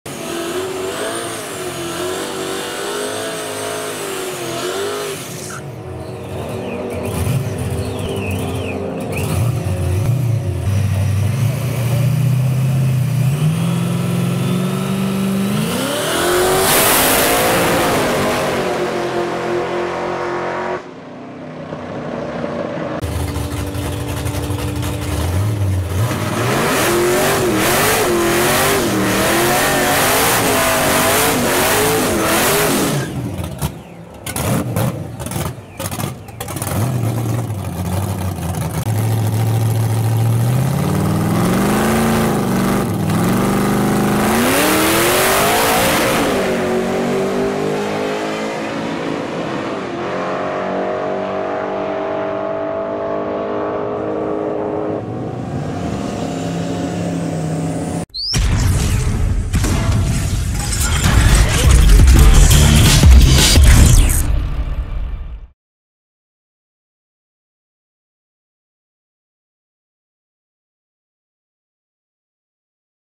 TUFF 9 SEC SUPERCHARGED STREET DRIVEN VN ON DEBUT
TUFF 9 SEC SUPERCHARGED STREET DRIVEN VN ON DEBUT - VORTECH SUPERCHARGED HOLDEN 304